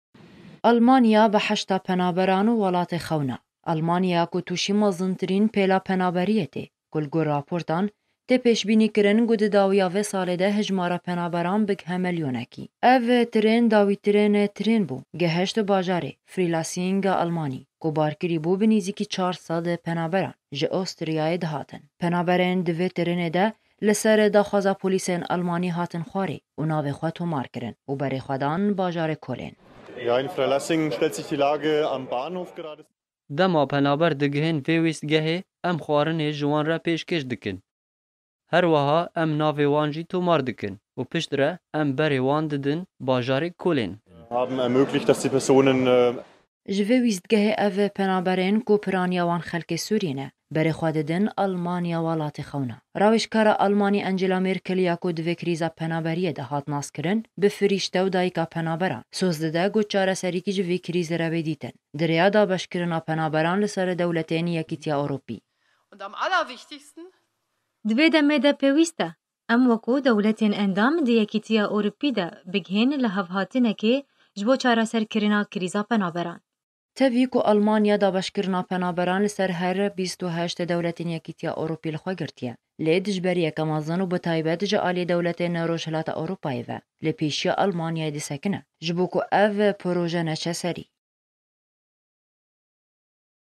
Elmanya ... Tirêneke hilgirtî bi 400 penaberan gihişt Elmanya
وصول قطار يقل 400 مهاجر إلى مدينة فريلاسنغ الالمانية